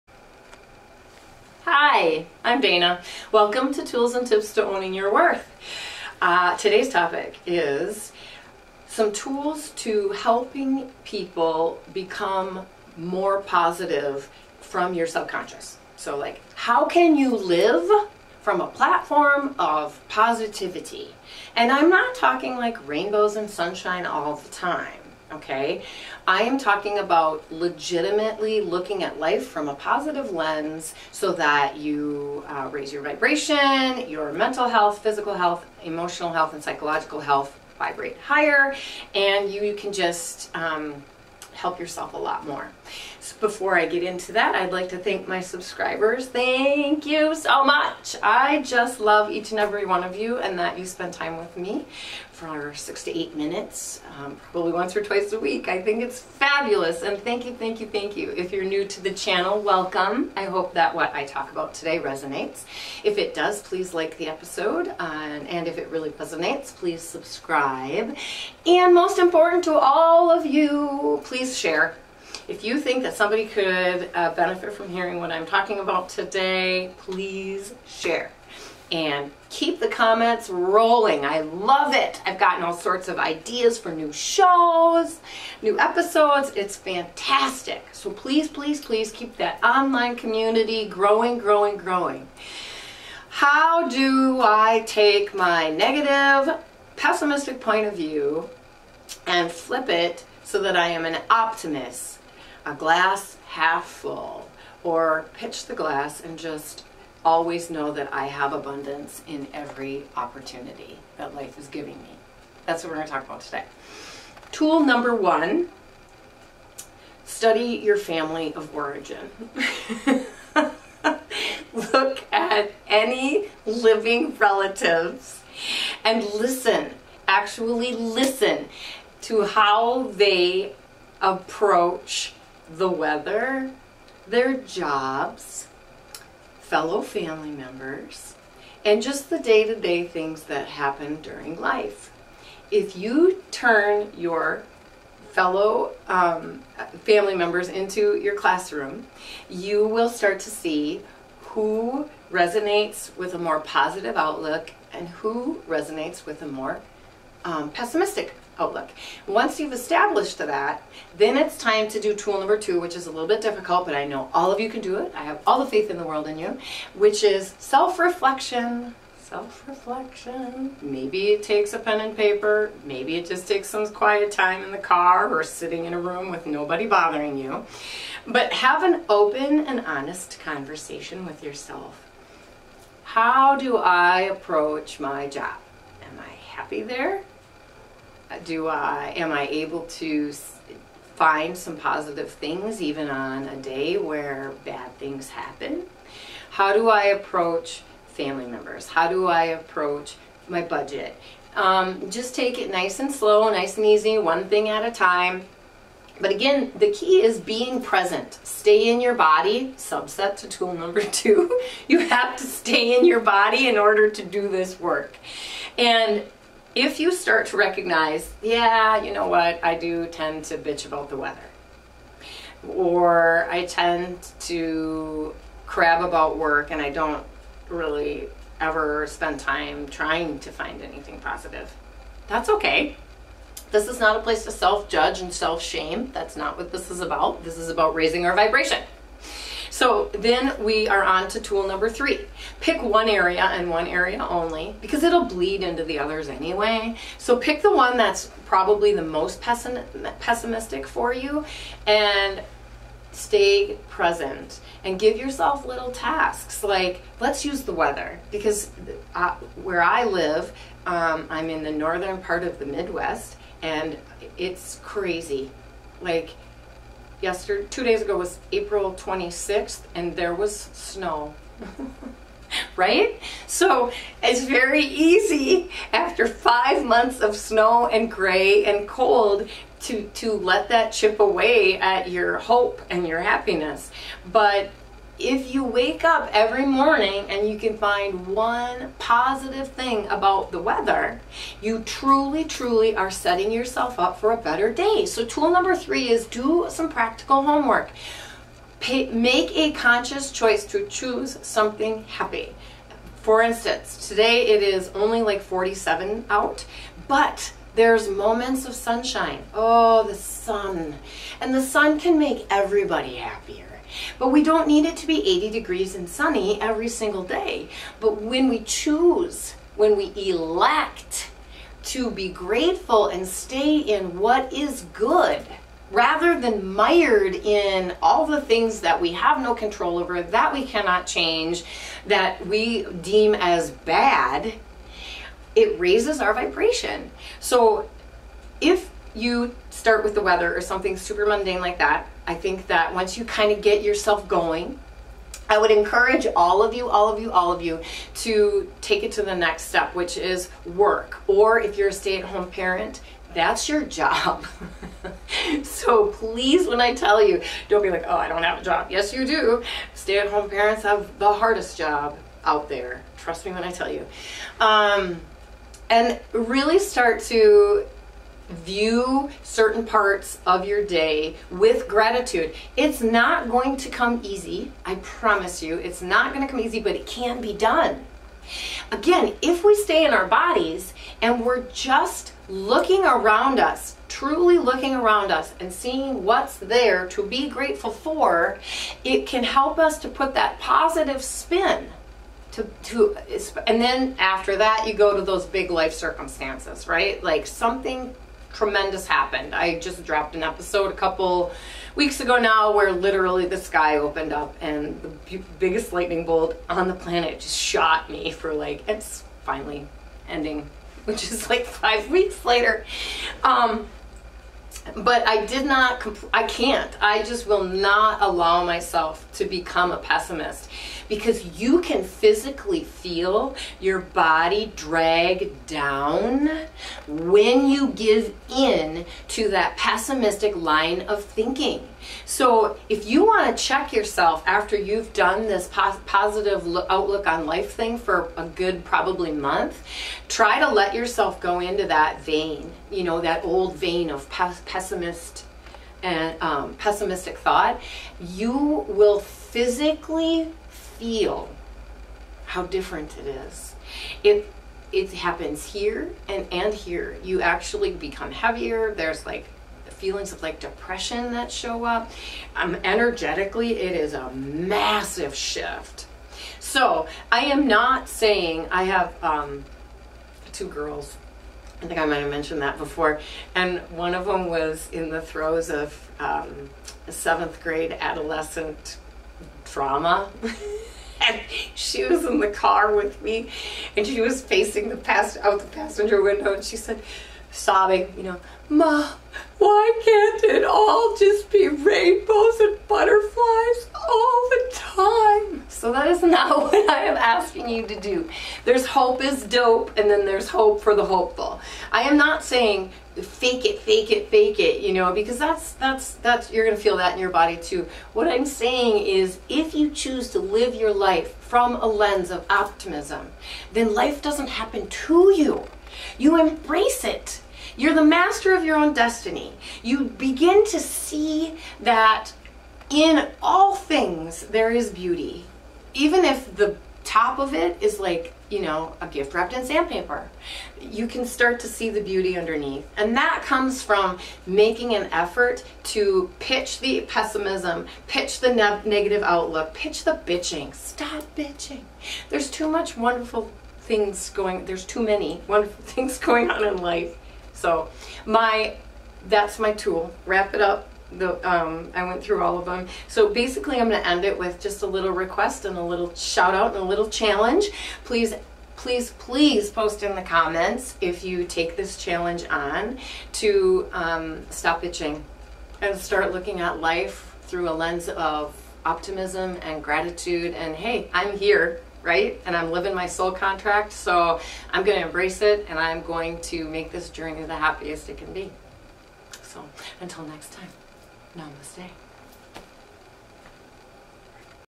Trading Your Negative Outlook for a Positive One Makes you Healthier...FOR SURE!
I would like to point out that I am getting over a pretty decent energetic uplevel so I apologize for my creaky voice. Having a cold and understanding that your body is healing itself from suppressed emotions with the cold is great, but doesn't make the end result any different...still sick HA!

This is the link to my email. I realize some people don't feel comfortable posting comments on a public forum. I get that.

Also! Apparently I am shouting out my favorite coffee house in the video unexpectedly as well! HAHA. Gotta love me some LAWLSS coffee even though having my cup up was unintended 🧋

I have linked the Please Be Grateful episode here as well, as these two definitely go hand in hand.

Good luck with your own Optimist challenge and like I said in the video, PLEASE comment on where you are choosing to focus your attention and on your progress. I am your BIGGEST cheerleader for sure! So let me know how you're doing!!! 🌈🦄🦋